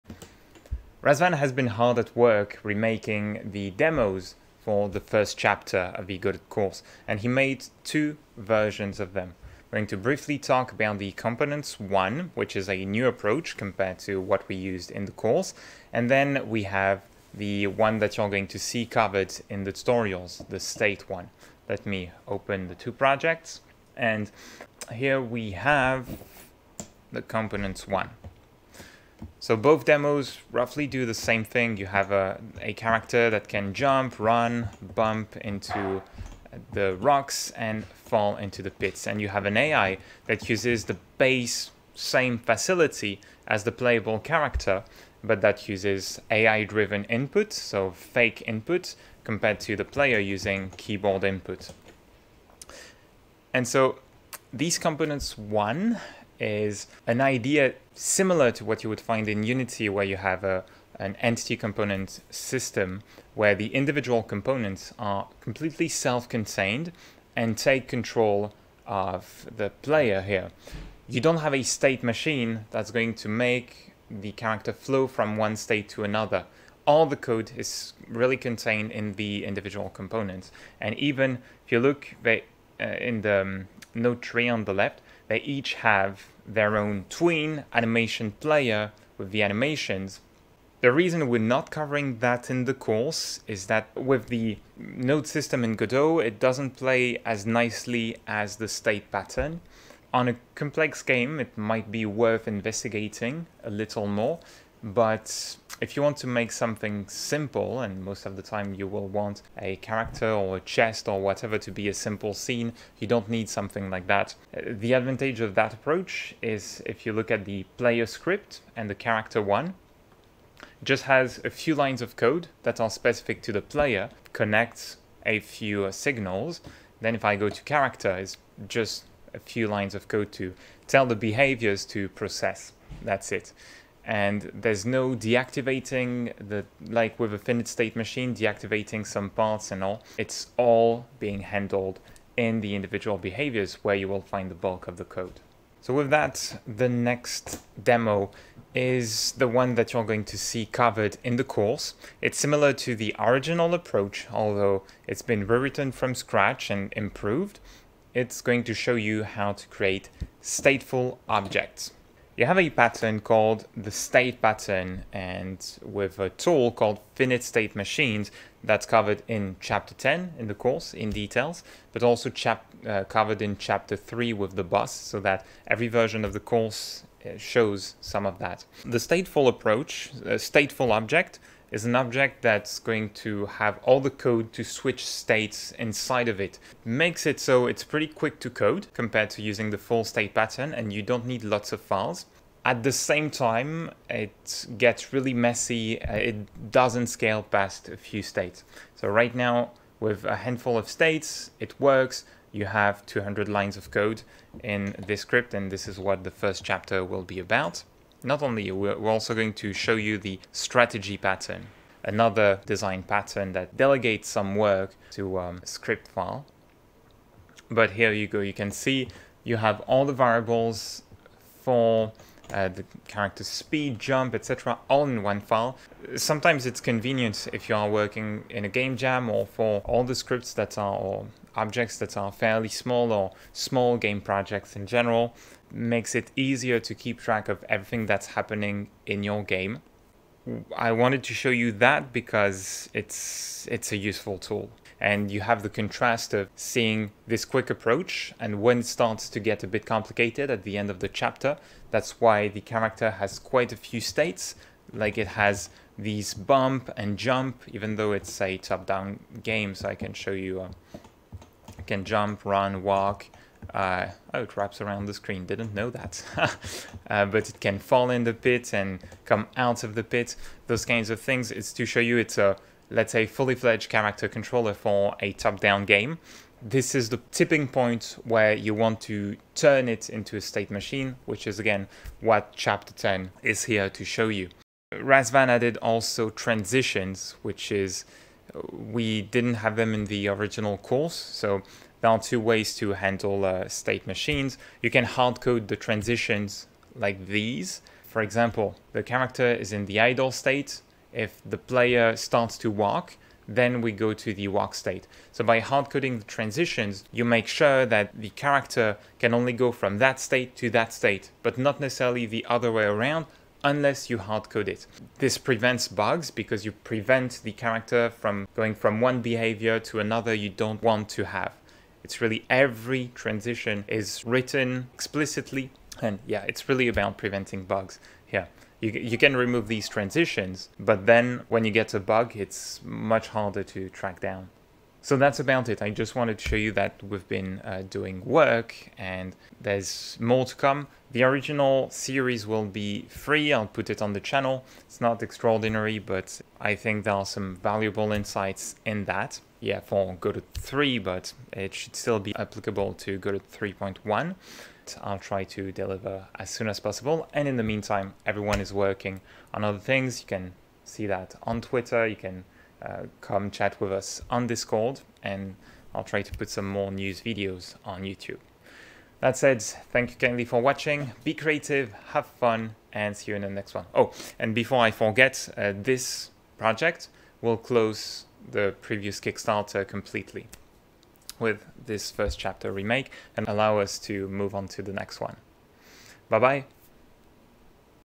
Quick Look: Demo remake for the Godot course chapter 1
A quick look at the two demos Razvan made for the remake of first chapter of our previous Godot course, Make Professional 2d Games with Godot